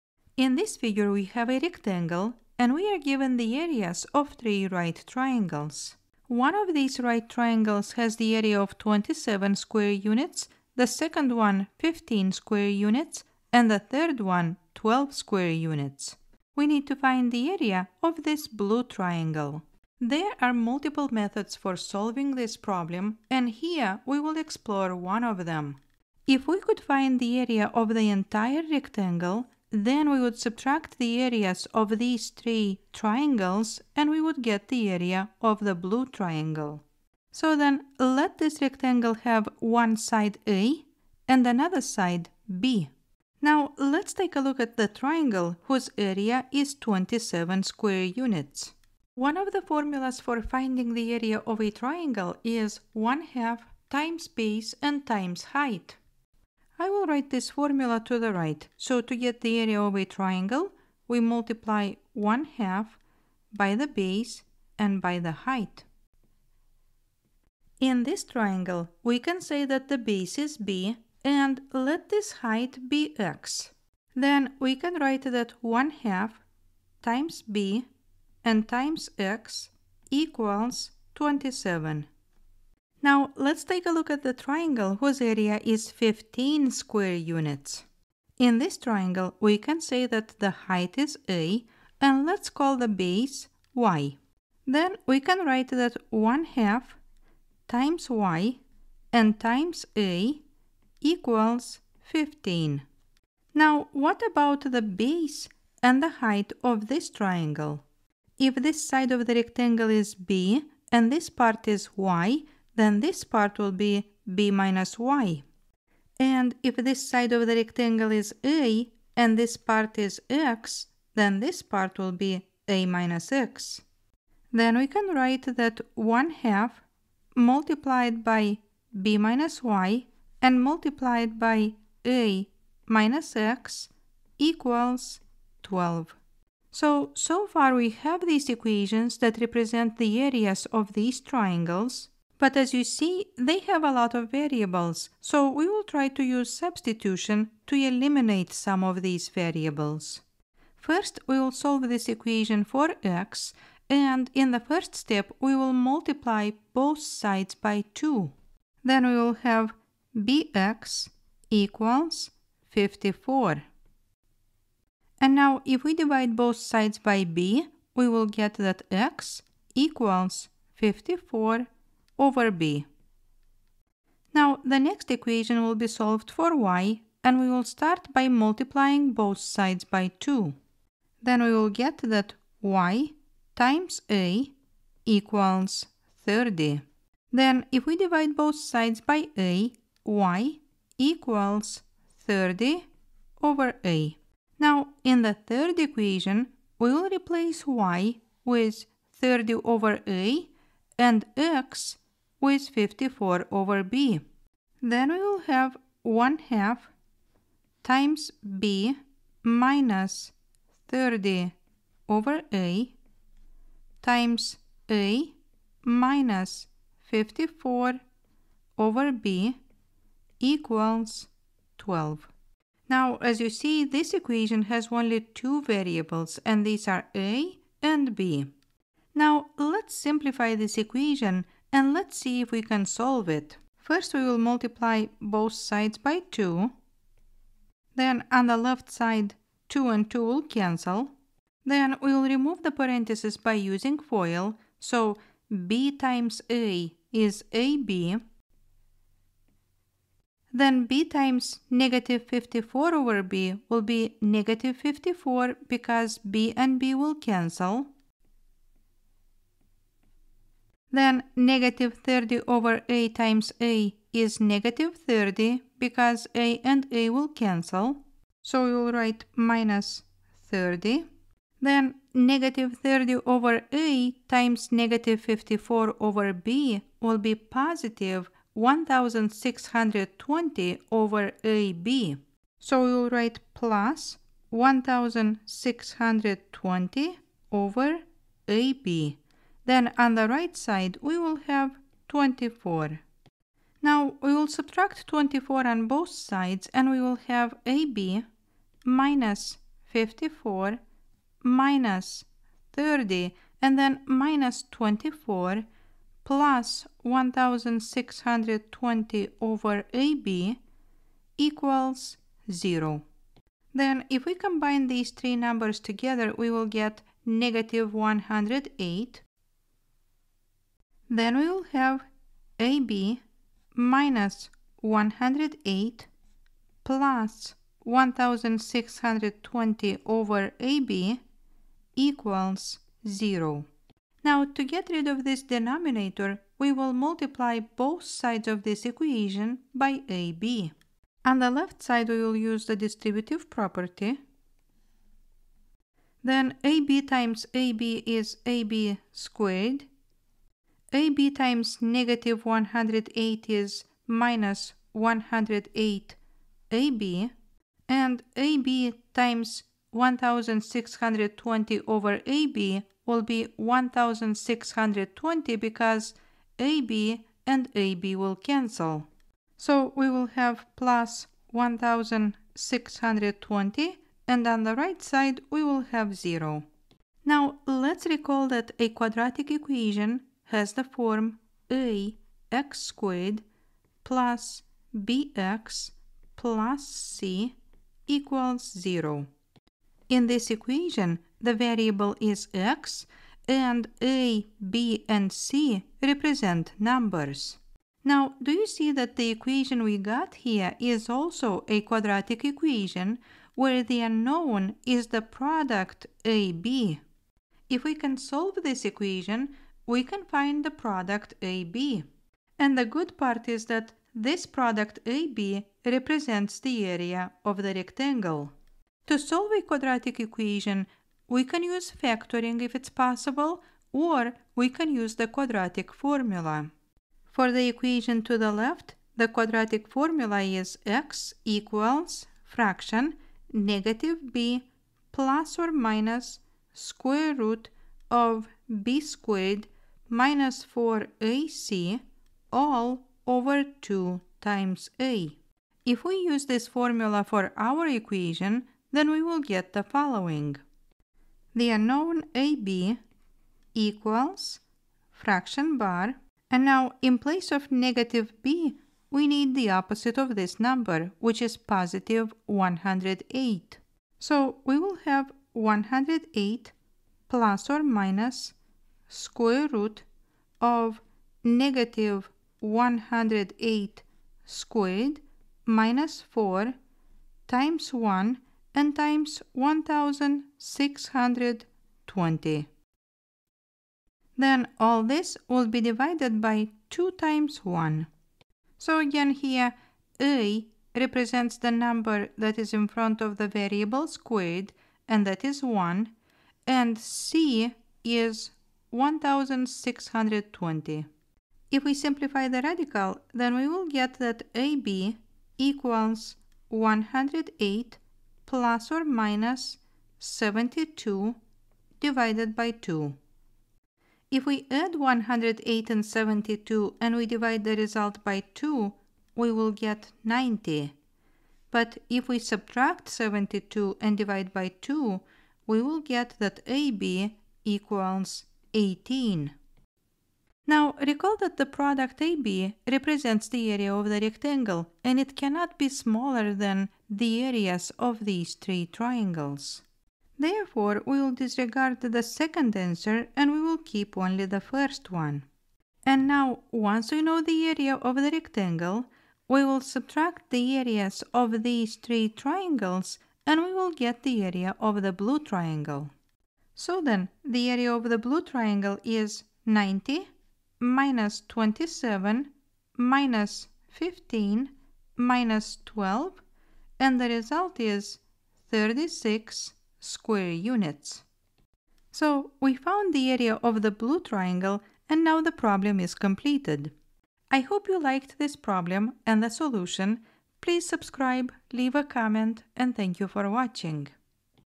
Find the Area of the Blue Triangle Inside of a Rectangle
In this video we use the formulas for the areas of rectangles and triangles, as well as the quadratic formula to find the area of the blue triangle.